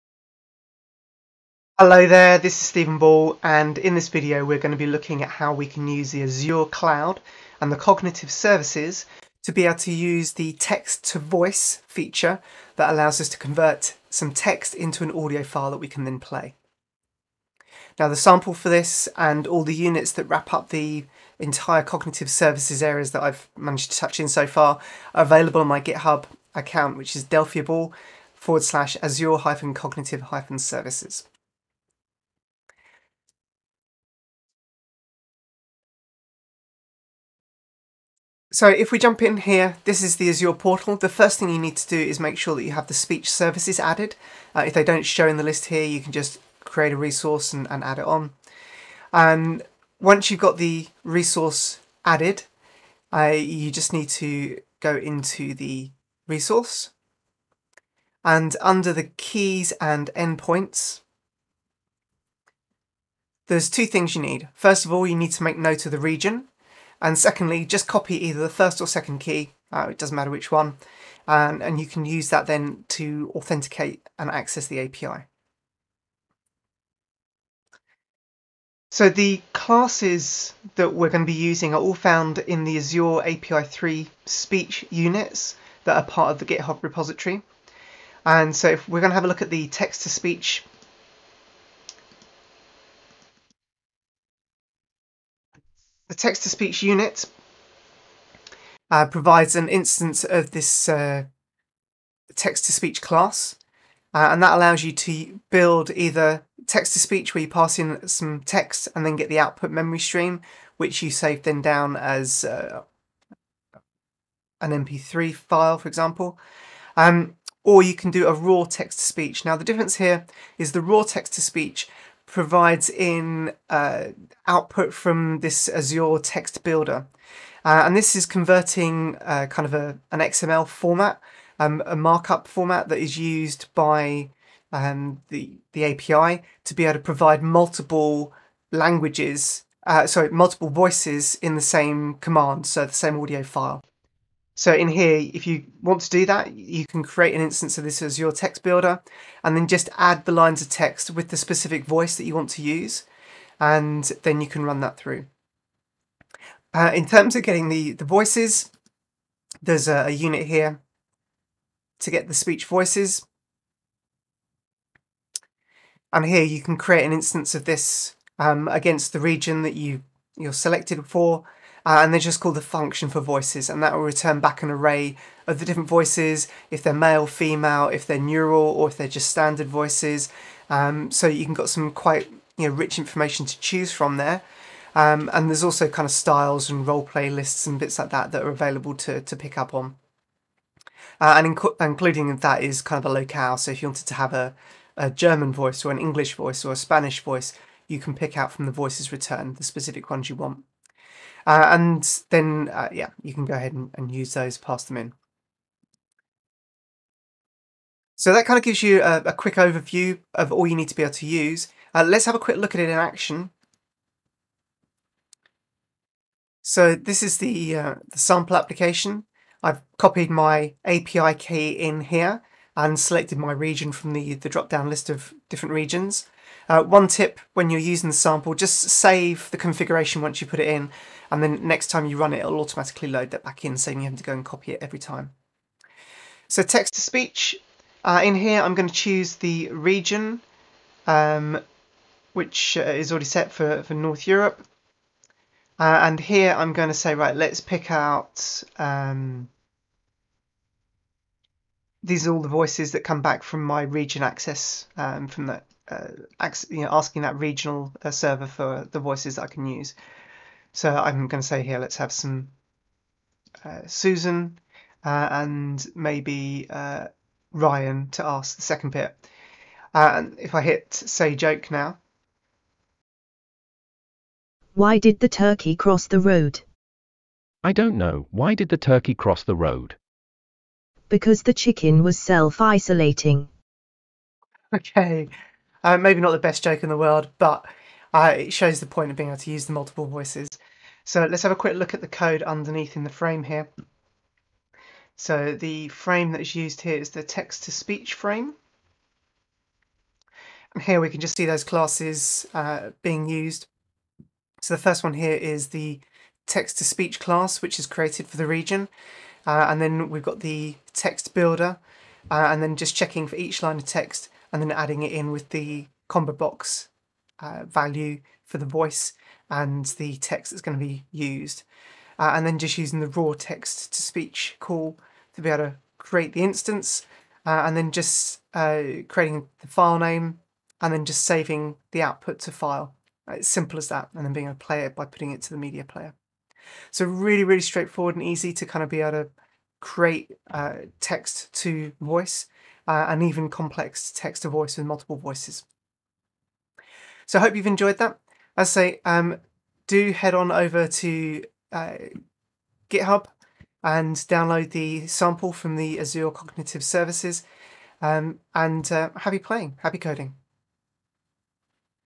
Using Azure Cognitive Services - Text To Voice
How to use the Azure Cloud Cognitive Services to convert text into audio. Using Delphi its simple and easy thanks to open source wrapper classes you can download from Github

Table of Contents:

00:00 - Speech Service &gt; Introduction
00:24 - Speech Service &gt; Text to Voice on Github
00:37 - Speech Service &gt; Setup and access key in Azure Portal
01:17 - Speech Service &gt; Looking into the code
03:35 - Speech Service &gt; Running the sample
05:48 - Speech Service &gt; Close